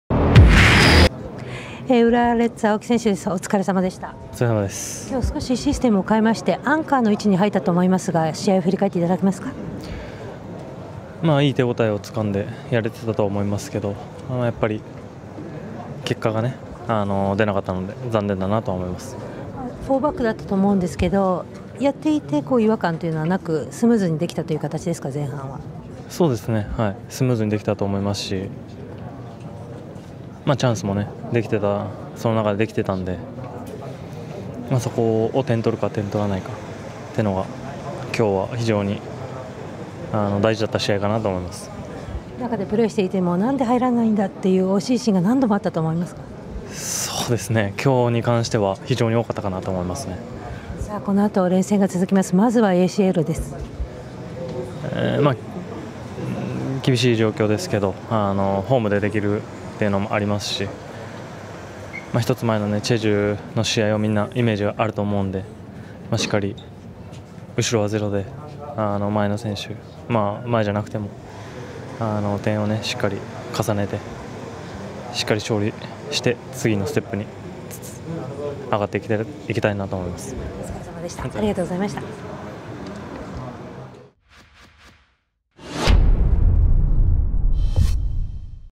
【公式】試合後インタビュー動画：青木 拓矢（浦和）浦和レッズvs柏レイソル 明治安田生命Ｊ１リーグ 第25節 2017/9/9
2017年9月9日（土）に行われた明治安田生命Ｊ１リーグ 第25節 浦和vs柏の試合後に収録された青木 拓矢（浦和）のインタビュー動画です。